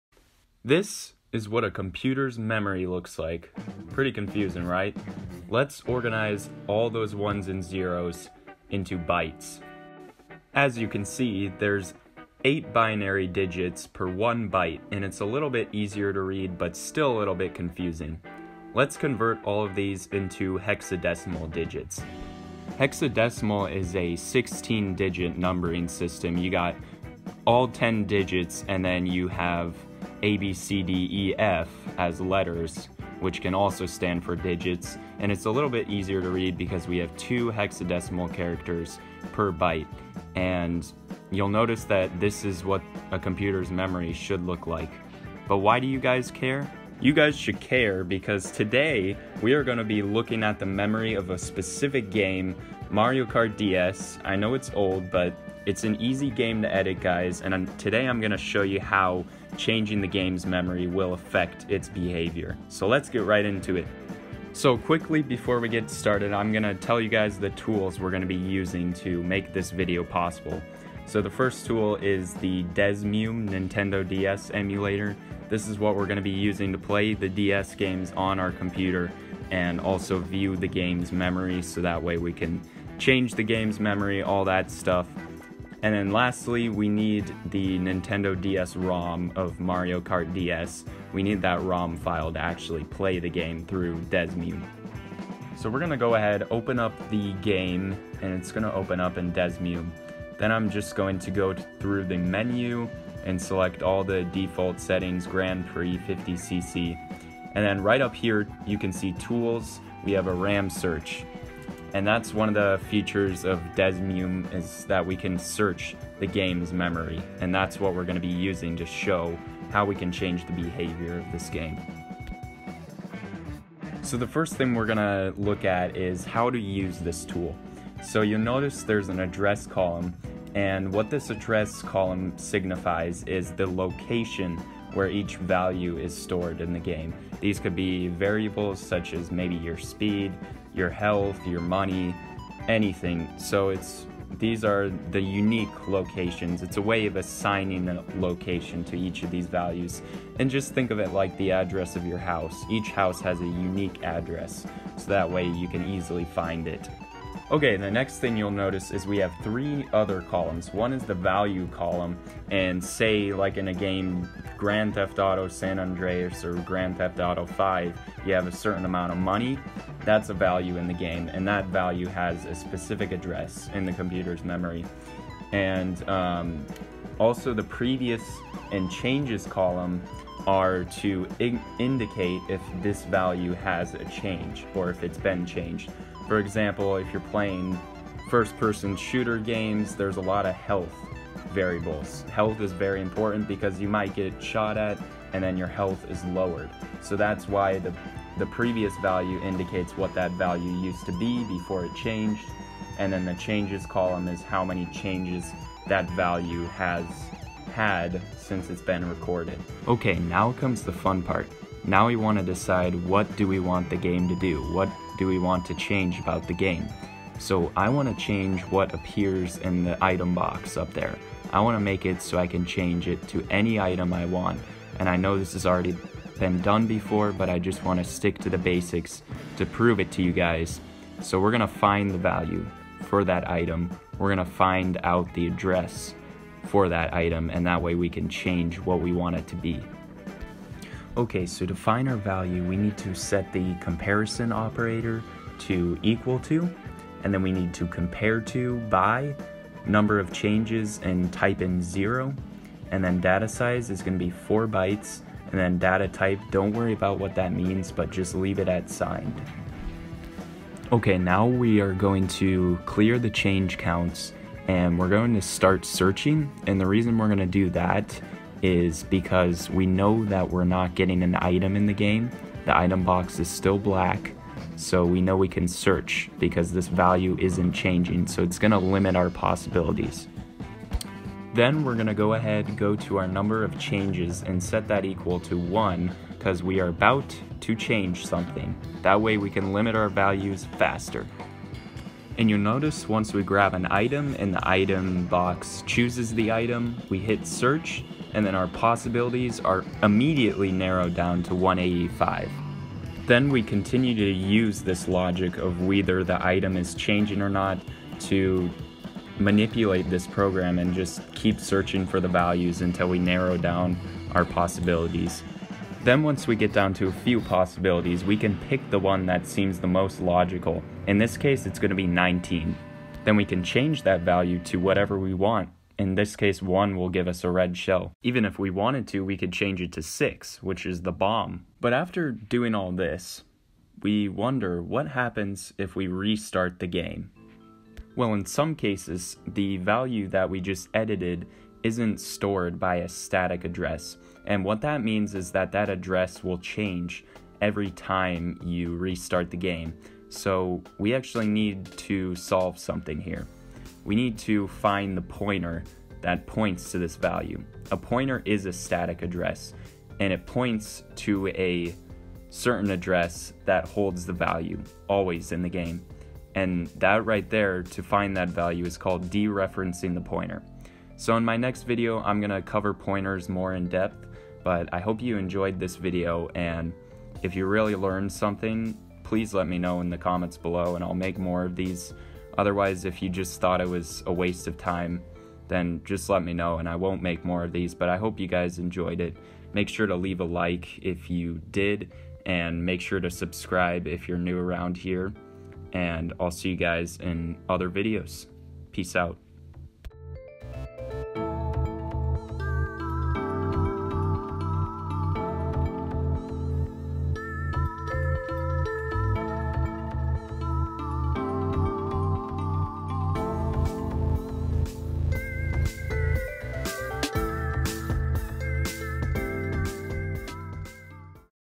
How Does Computer Memory Work?
Welcome back to another episode of EV Music! I know this video isn’t music related, but it is technology related, and that is part of what I like to cover on this channel. In today’s video, I will be showing you how binary and hexadecimal are used to store software on a computer. In this specific video, we will be looking at using memory tools to hack games in order to have them do what we want them to do.

Other computer memory tools and concepts will be introduced in future videos.

-Evan